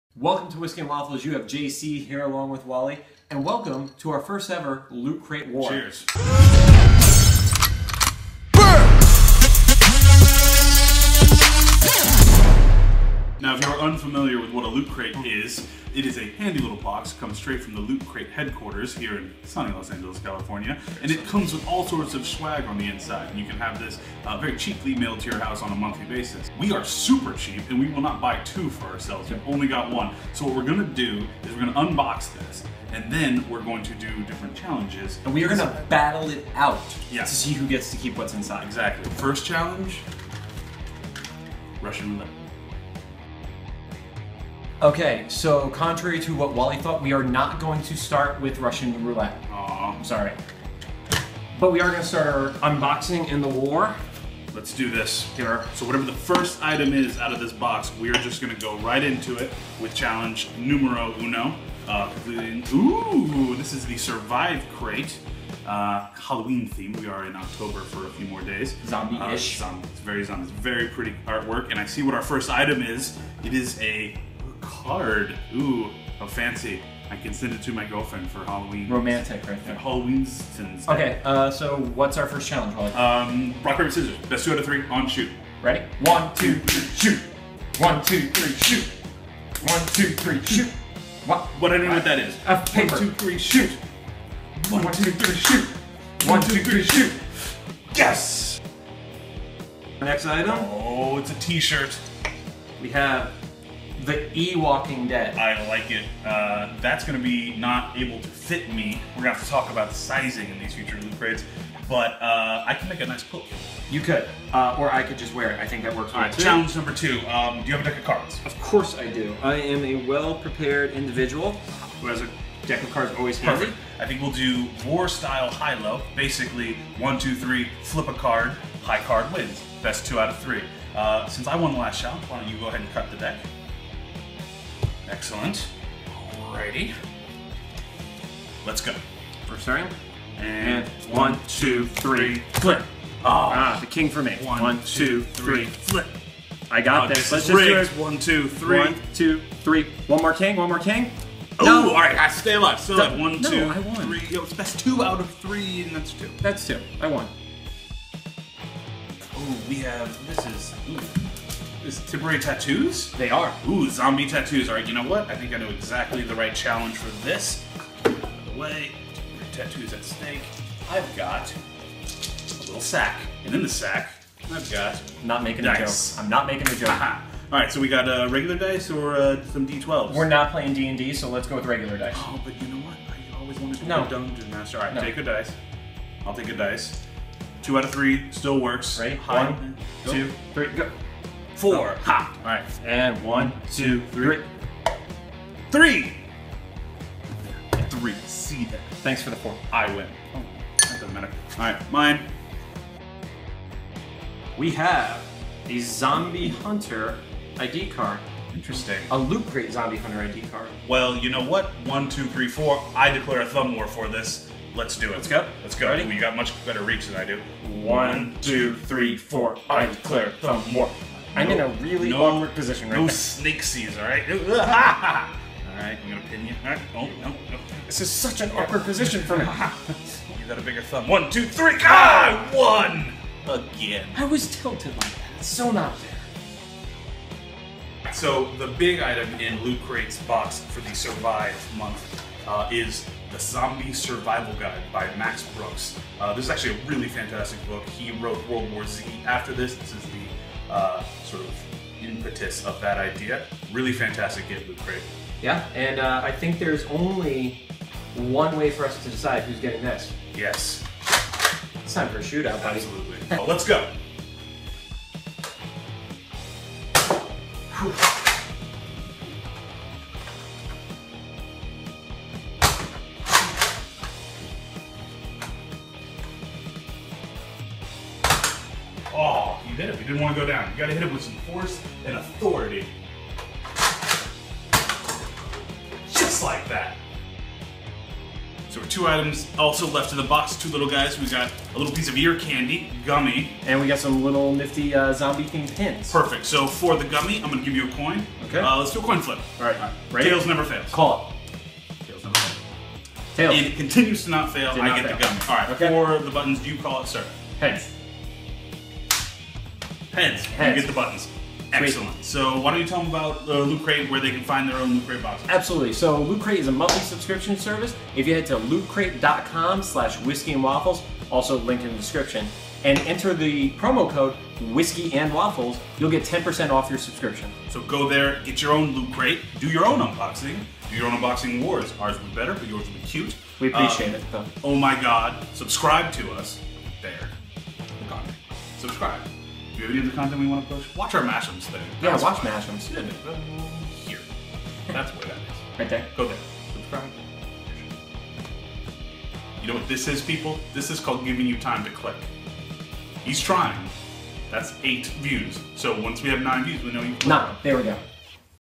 Zombies and the Ewoking Dead - Loot Crate Wars: Survive Unboxing - Whiskey and Waffles
Introducing Loot Crate Wars where we battle it out for each and every item in a Loot Crate box. Join WallE and JC as they unbox October's Loot Crate Survive, featuring Zombies and the Ewoking Dead!

Have a suggestion for a video you want to see us make?